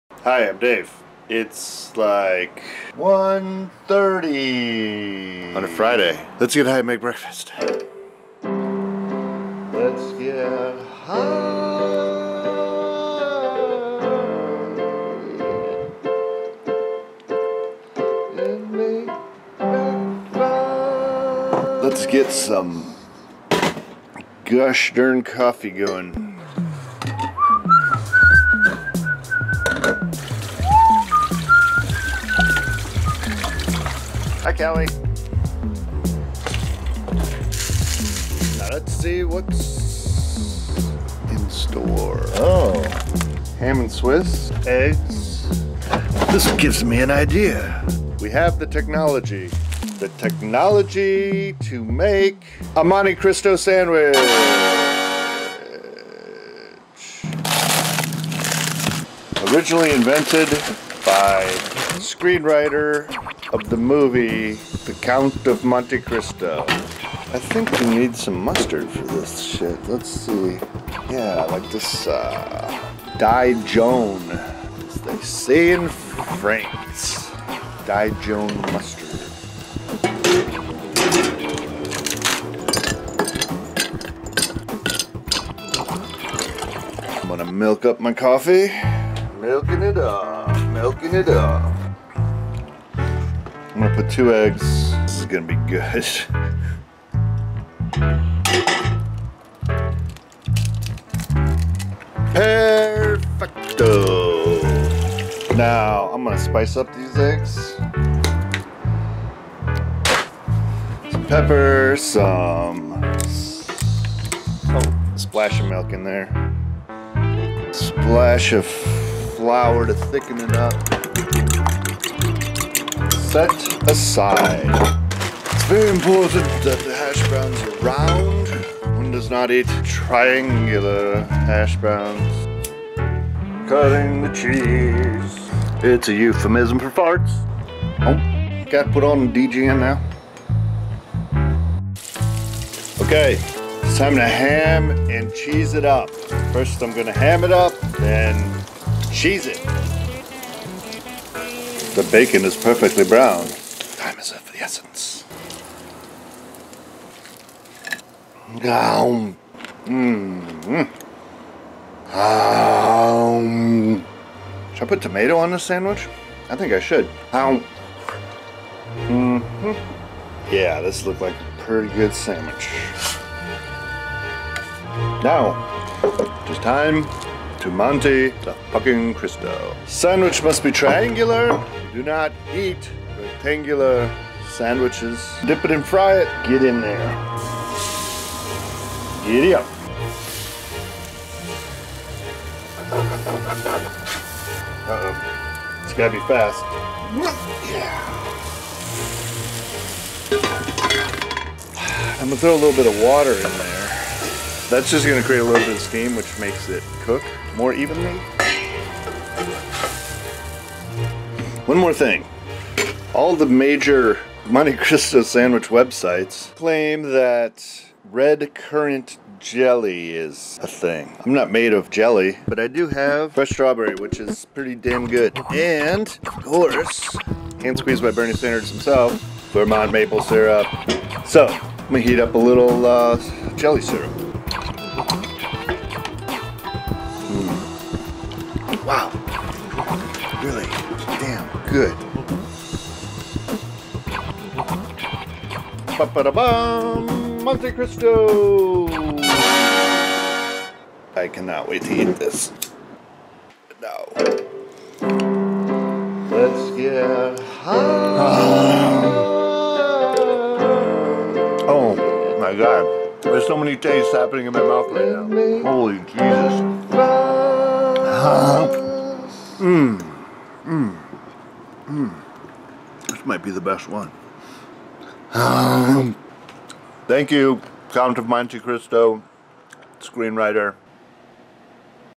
Let's Get High and Make Breakfast - Monte Cristo!
In which I smoke a bowl of Snow White sativa and make a Monte Cristo sandwich, drenched in strawberry maple syrup with hash browns.